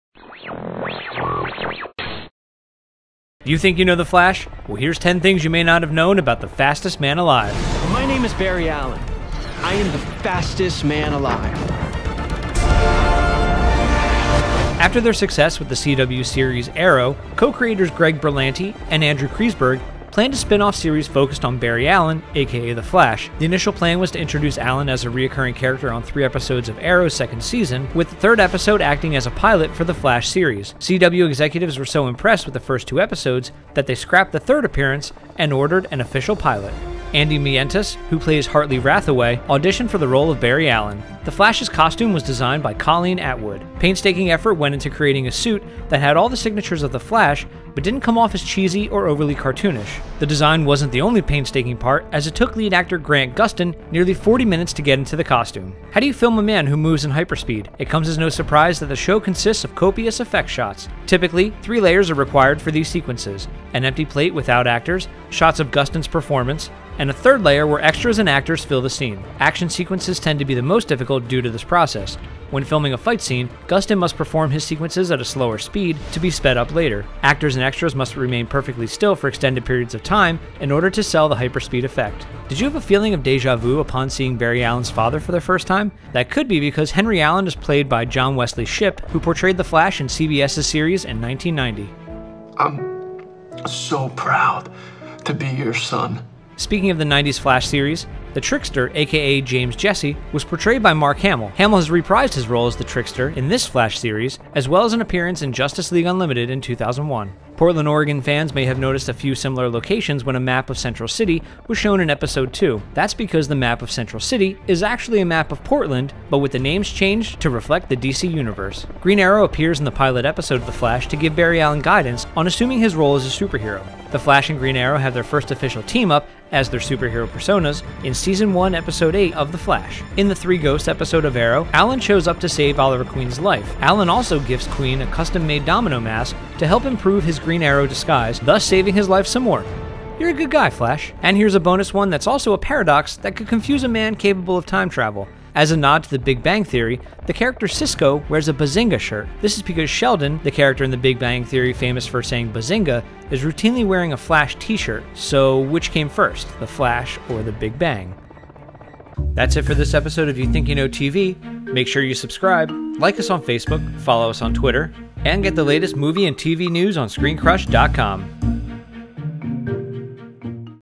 The Flash - You Think You Know TV?
Did you know it takes The Flash star Grant Gustin over 45 minutes just to get into his costume every day? Find out even more Flash facts in our video about The Fastest Man Alive!